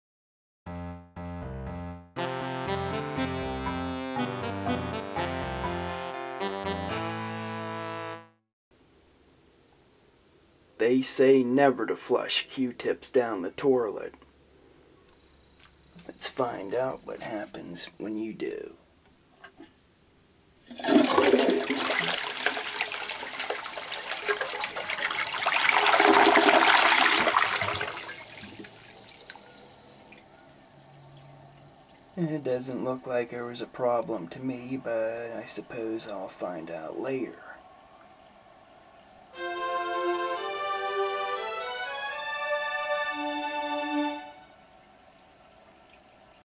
Will It Flush? - Q-Tips
Episode 74: Q-Tips

(Rated G) You've heard that you're not supposed to flush Q-Tips, I presume? Well, I flushed some so you don't have to. I expected a Q-tip to get lodged and clog the toilet but thankfully it never happened. We're coming near the end of season 4 of this show, and I'll try to present something cool for the season finale.